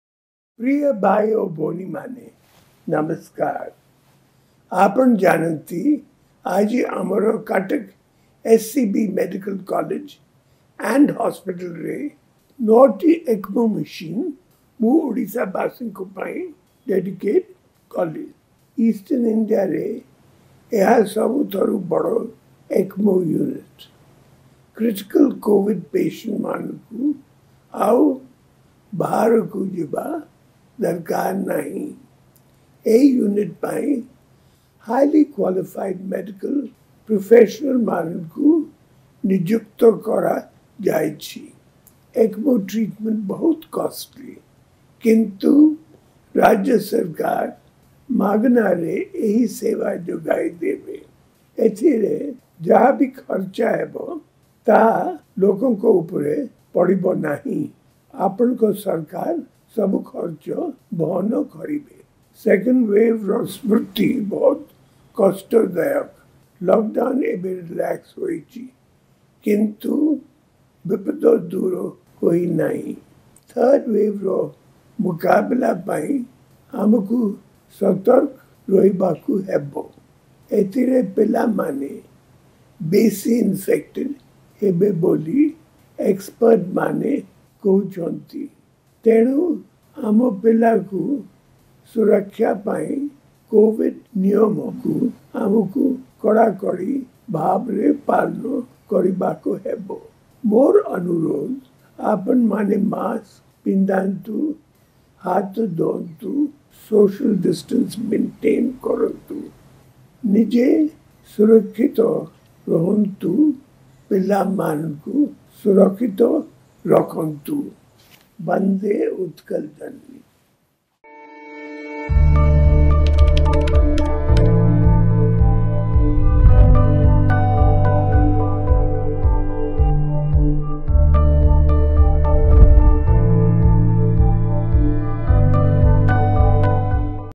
SCBରେ ଯୋଡି ହେଲା ନୂଆ ଅଧ୍ୟାୟ, ଏଣିକି ମାଗଣାରେ ହେବ ECMO ଚିକିତ୍ସା | ecmo machine inaugurated by cm
ଚିକିତ୍ସା କ୍ଷେତ୍ରରେ ରାଜ୍ୟକୁ ମିଳିଲା ନୂଆ ଉପଲବ୍ଧି । ରାଜ୍ୟରେ ଆରମ୍ଭ ହେଲା ଏକମୋ ସେବା । ଶୁଭାରମ୍ଭ କଲେ ମୁଖ୍ୟମନ୍ତ୍ରୀ